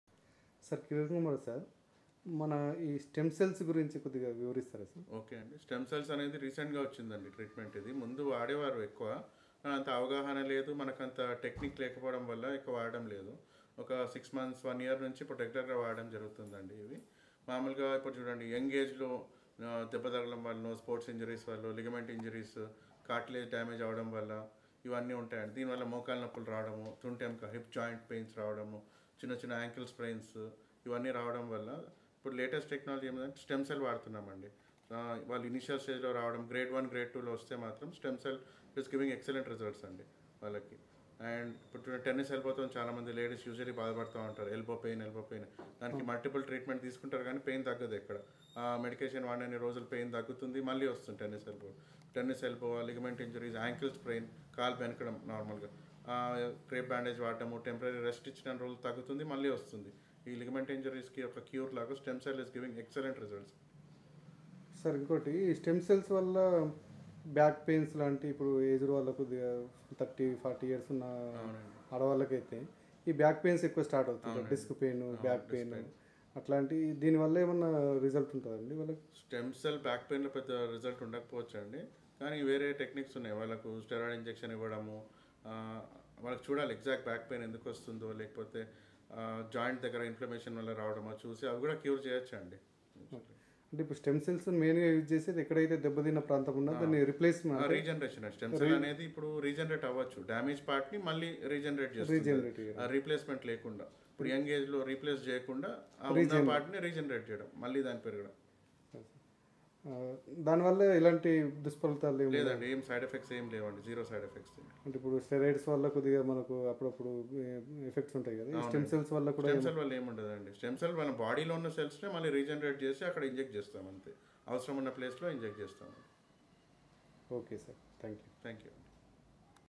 Interview with Dr Kiran kumar Reddy Gowni Ms(Ortho) Mch (Ortho) Discuss about Stem Cell's
Dr Kiran Kumar Reddy Gowni
Ms(Ortho) Mch (Ortho)

Fellow in Joint Replacement Surgery (NJ,USA)
Fellow in Joint Replacement Surgery (Singapore)
Spl in Complex Trauma, Arthroscopy and Joint Replacement Surgeon
Yashoda hospital somajiguda.

TOTAL_KNEE_REAPLACEMENT surgery in YASHODA HOSPITAL SOMAJIGUDA.